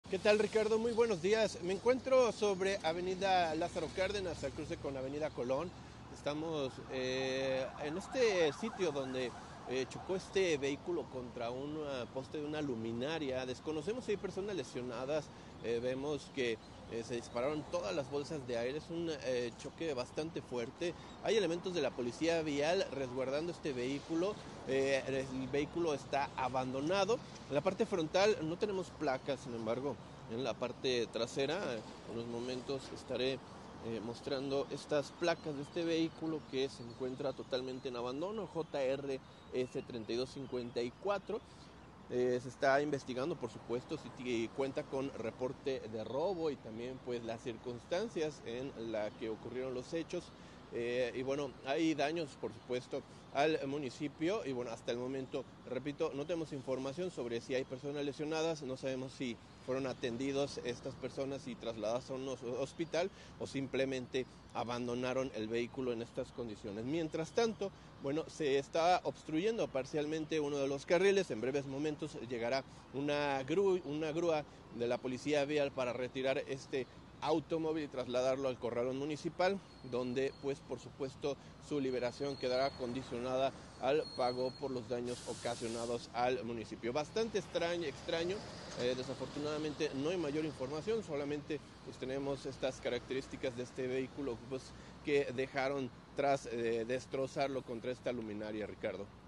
Abandonan vehículo tras accidente en Av. Colón | Noticias GDL con Ricardo Camarena
Abandonan vehículo tras accidente en Av. Colón y Calzada Lázaro Cárdenas.